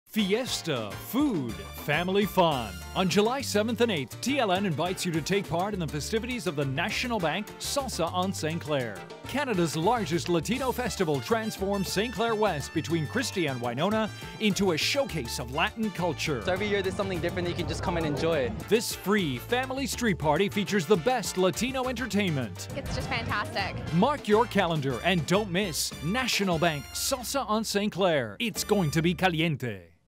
National Bank Salsa on St Clair Promo
8th Annual National Bank Salsa on St. Clair
Saturday July 7th, 2012 (From noon -- until 11pm)
Sunday July 8th, 2012 (From noon -- until 8pm)

TLN Telelatino, the Hillcrest Village BIA and the City of Toronto have once again joined forces to present the second stop on the summer tour, National Bank Salsa on St. Clair, Canada's largest Latino-themed street festival which wraps up Latino week in Toronto.

National Bank Salsa on St. Clair has attracted over 1.5 million salsa lovers to the streets of Toronto since its launch in 2005.

Join us for the 8th annual National Bank Salsa on St. Clair on July 7th and 8th 2012 as midtown Toronto's Village from Winona Drive to Christie Avenue is transformed into a showcase of Latin life featuring live music, dance lessons, fiery foods and family fun!